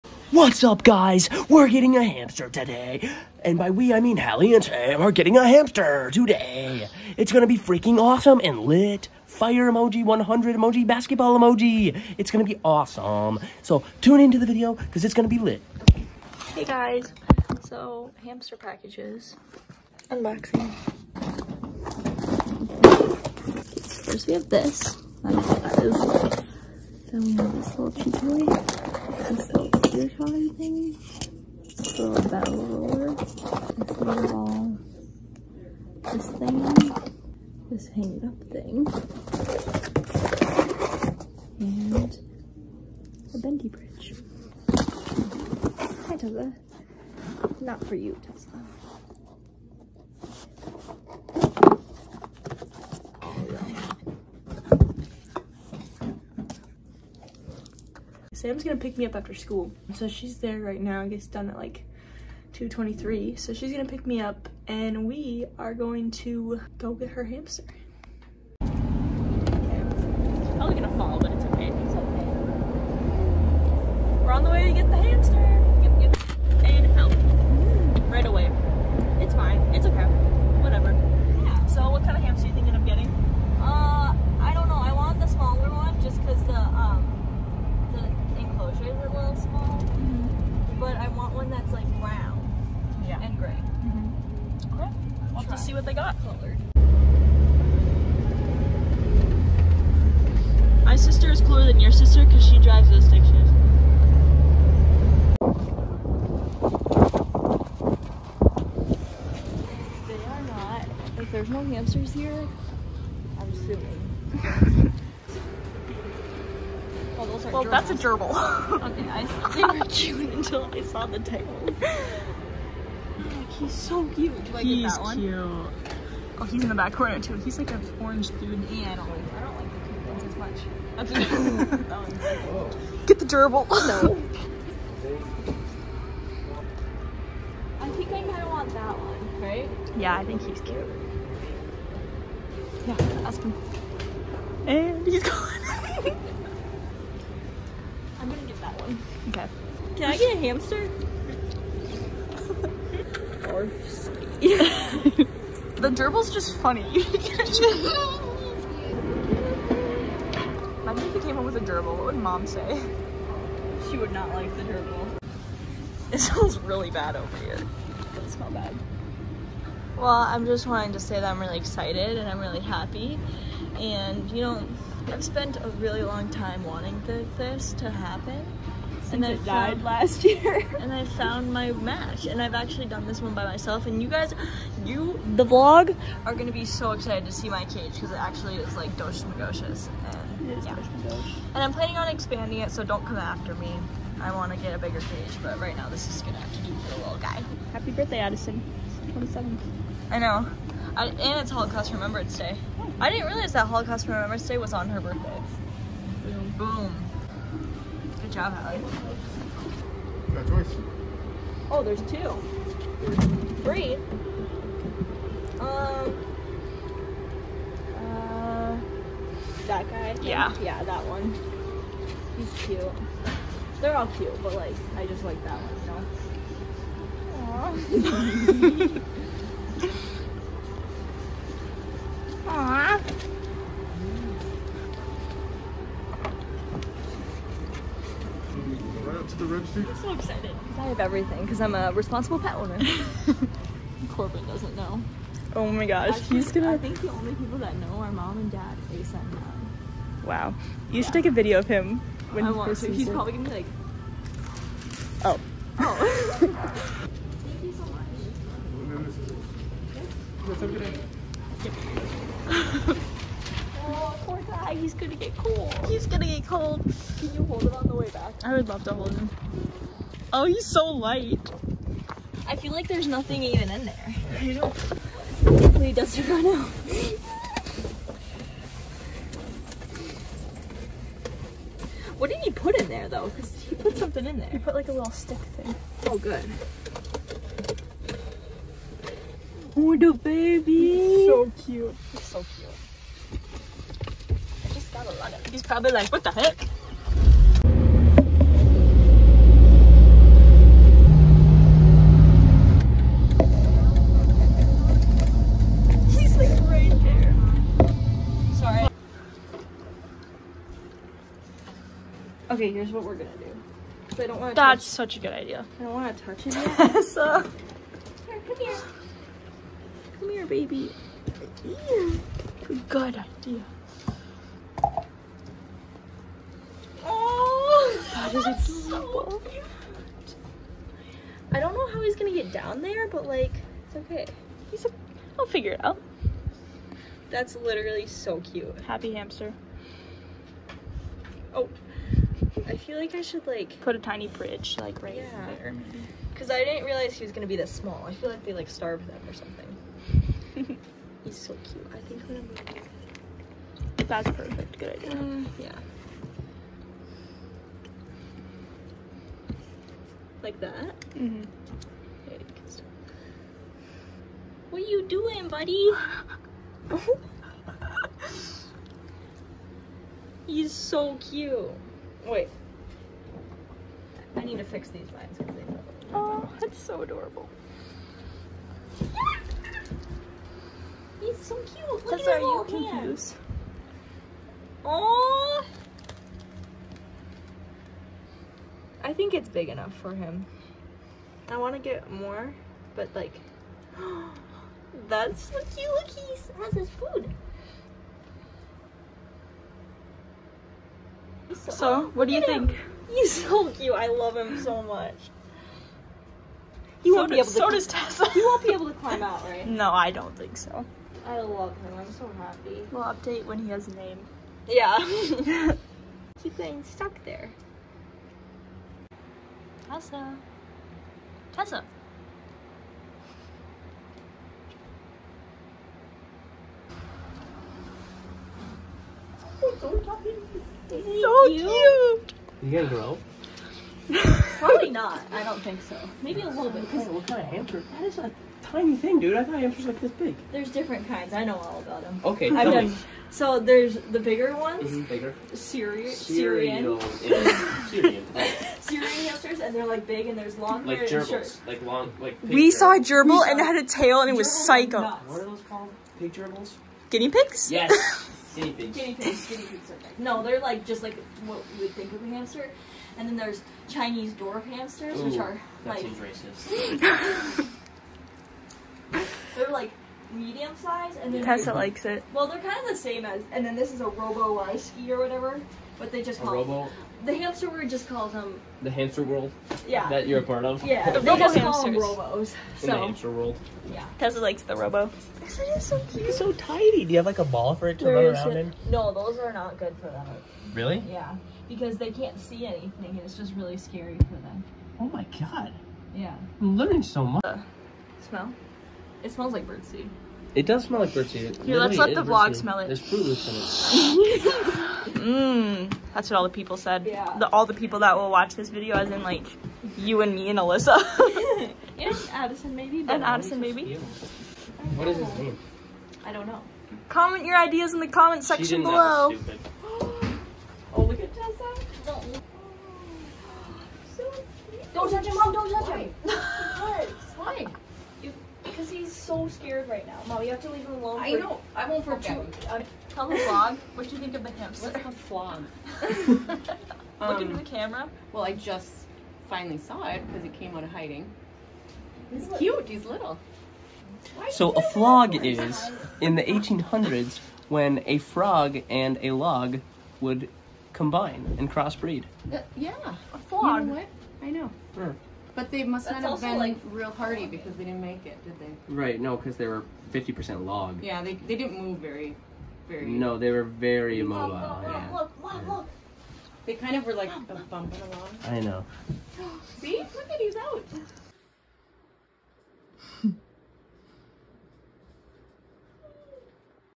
Sam gets a hamster!
Sam and I go and get a hamster yayyy!

Sam did have a hamster last year around December, but it died. This time she ordered and got everything ready and it has a wayyy bigger area than before. She was so excited!

1•27•23